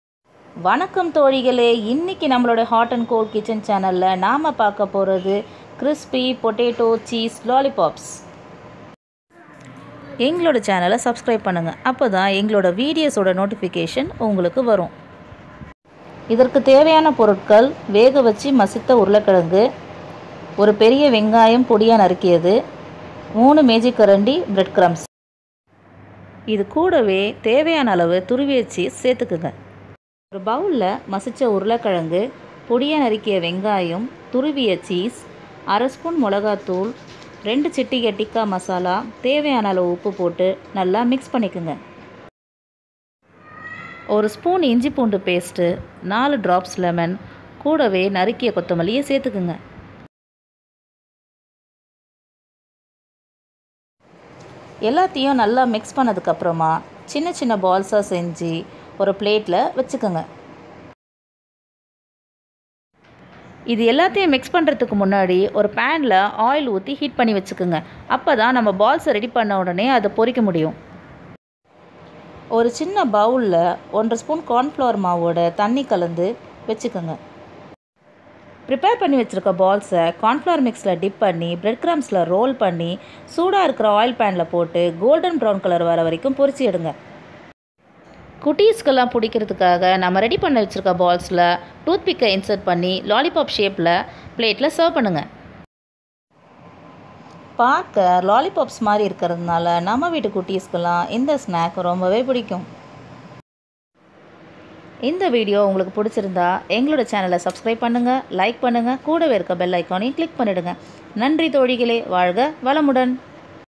3 பொருள் போதும்... மொறு மொறு உருளை சீஸ் லாலிபாப்ஸ் தயார்/ Crispy potato cheese lollipops ready..
3 பொருள் போதும் ..மொறு மொறு உருளை சீஸ் லாலிபாப்ஸ் தயார்/ Crispy potato cheese lollipops ready..